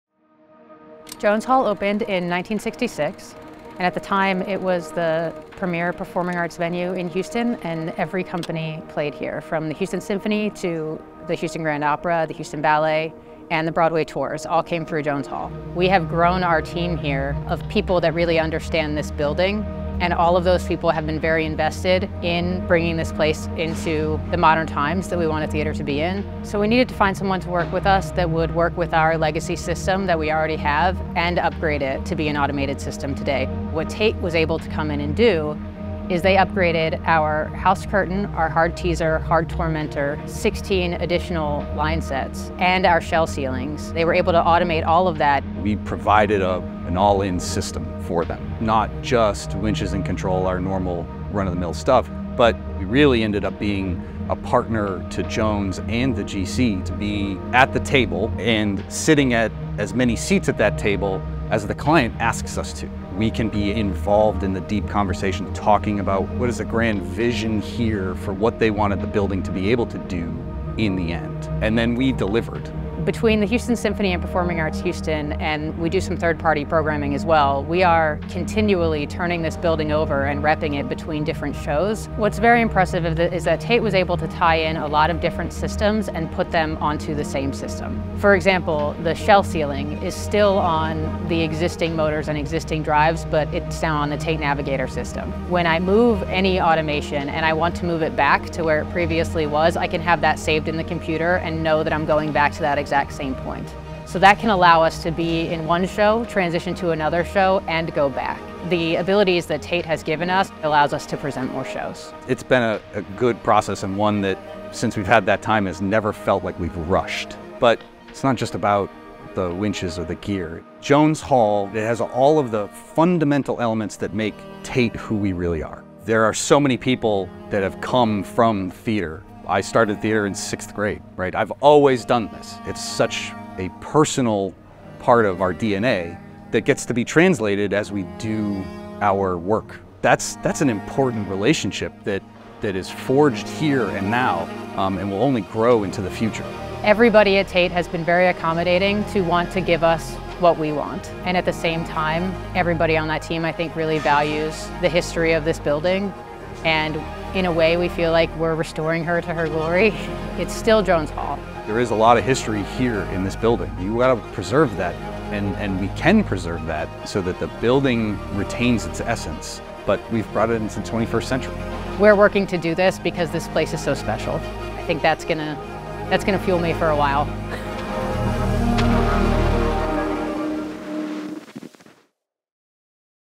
TAIT x Jones Hall Houston
Jones Hall, in Houston TX, partnered with TAIT for an upgrade of their stage automation machinery and controls, increasing the operational efficiency of this much loved venue.